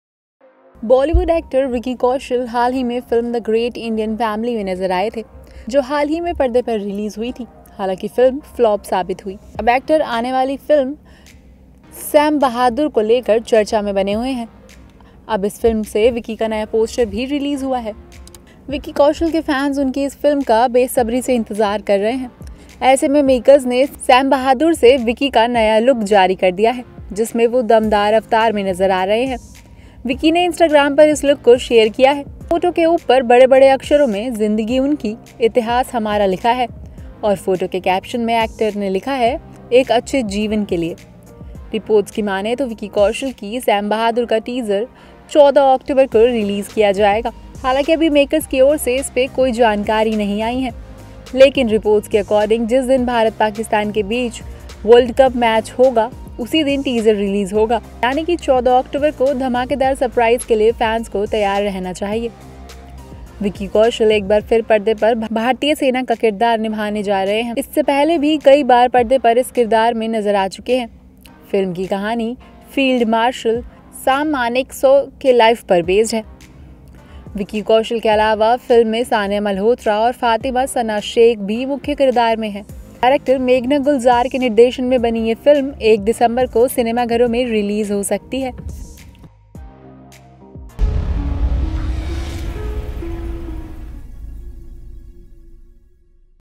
Vicky Kaushal ने Sam Bahadur की नई फिल्म का शानदार पोस्टर जारी किया
विक्की कौशल की मोस्ट अवेटेड फिल्म सैम बहादुर का कल टीजर रिलीज होने जा रहा है. उससे पहले फिल्म का नया पोस्टर सामने आया है. इस पोस्टर में विक्की कौशल यूनिफॉर्म में दिखाई दिए हैं.

The teaser of Vicky Kaushal's most awaited film Sam Bahadur is going to be released tomorrow. Before that a new poster of the film has come out. Vicky Kaushal is seen in uniform in this poster.